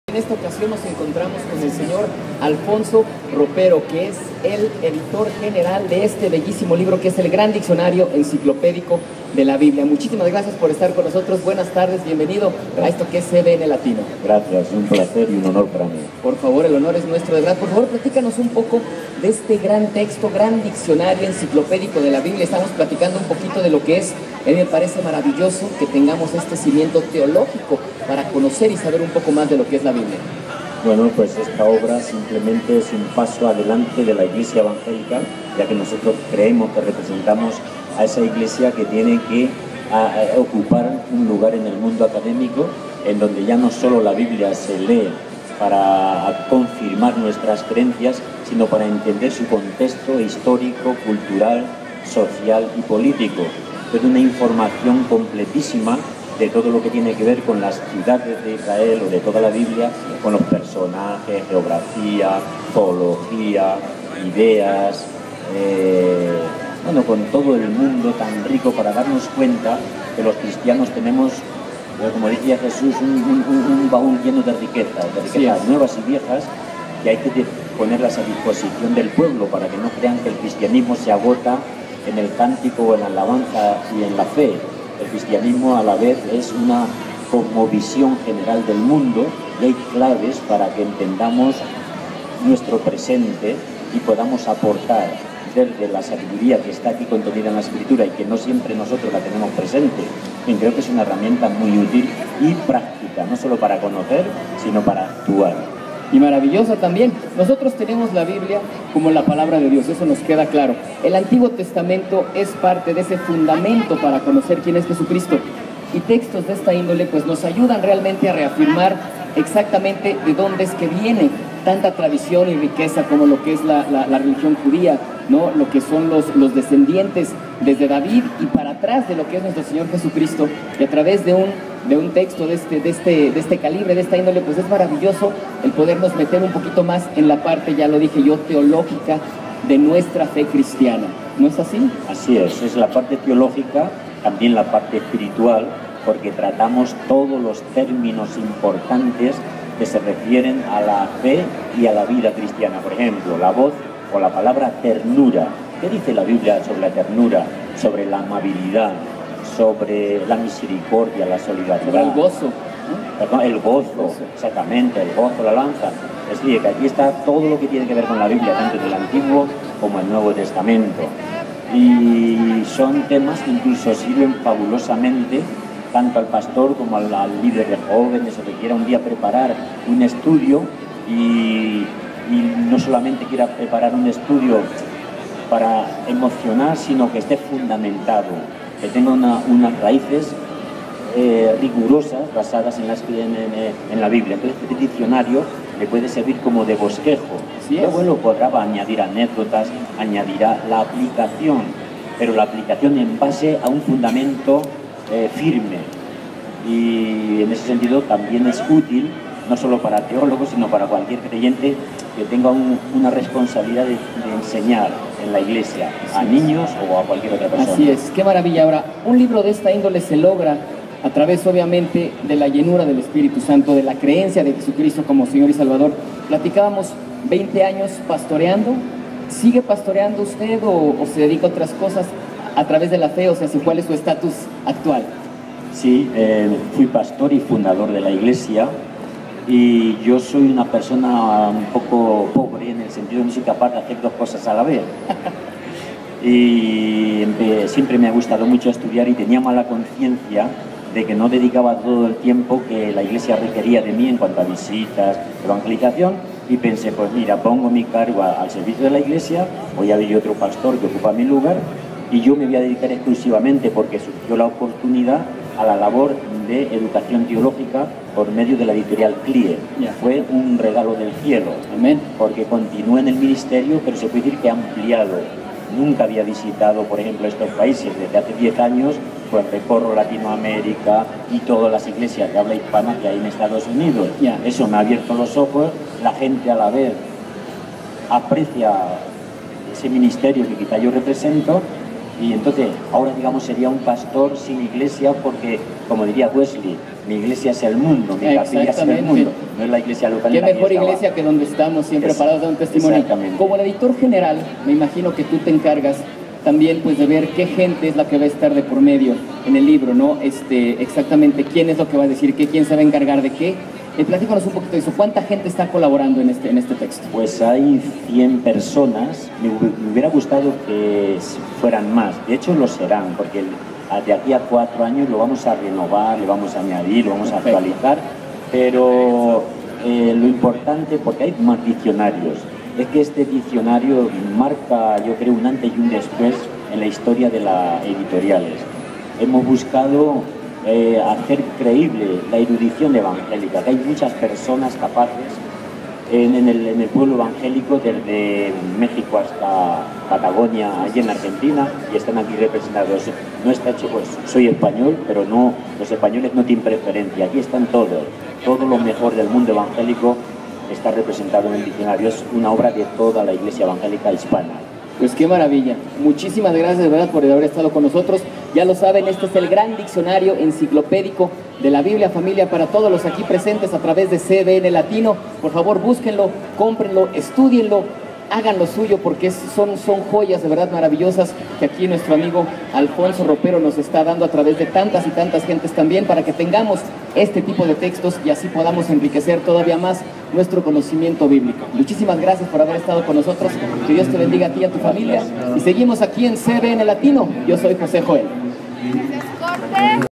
Alfonso Ropero en Expocristiana
El escritor español Alfonso Ropero, editor del Gran Diccionario Enciclopédico de la Biblia, en entrevista con José Joel luego de su participación en ExpoCristiana 2013.